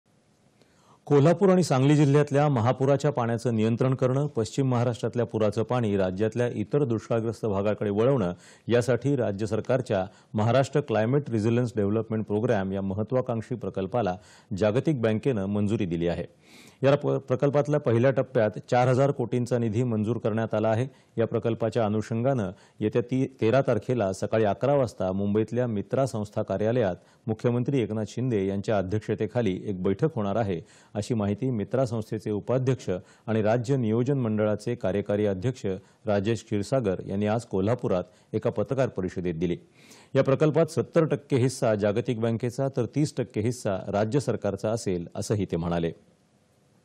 महाराष्ट्र क्लायमेट रिझीलिन्स डेव्हलपमेंट प्रोग्रॅम या महत्वाकांक्षी प्रकल्पाला मंजुरी.
दि. ११.०२.२०२४ :
   कोल्हापूर आणि सांगली जिल्हयातल्या महापूराच्या पाण्याचं नियंत्रण करणं , पश्चिम महाराष्ट्रातल्या पुराचं पाणी , राज्यातल्या इतर दुष्काळग्रस्त भागाकडे वळवणं यासाठी राज्य सरकारच्या महाराष्ट्र क्लायमेट रिझीलिन्स डेव्हलपमेंट प्रोग्रॅम या महत्वाकांक्षी प्रकल्पाला जागतिक बँकेनं मंजुरी दिली आहे. या प्रकल्पाला पहिल्या टप्प्यात  ४ हजार कोटींचा निधी मंजूर करण्यात आला आहे. या प्रकल्पाच्या अनुषंगानं येत्या १३ तारखेला सकाळी ११ वाजता मुंबईतल्या मित्रा संस्था कार्यालयात मुख्यमंत्री एकनाथ शिंदे यांच्या अध्यक्षतेखाली एक बैठक होणार आहे, अशी माहिती मित्रा संस्थेचे उपाध्यक्ष आणि राज्य नियोजन मंडळाचे कार्यकारी अध्यक्ष राजेश क्षीरसागर यांनी आज कोल्हापुरात एका पत्रकार परिषदेत दिली. या प्रकल्पात ७० टक्के हिस्सा जागतिक बँकेचा, तर ३० टक्के हिस्सा राज्य सरकारचा असेल, असंही ते म्हणाले.

8.30am | 11.00am | 1.00pm | 4.30pm | 7.00pm | 9.30pm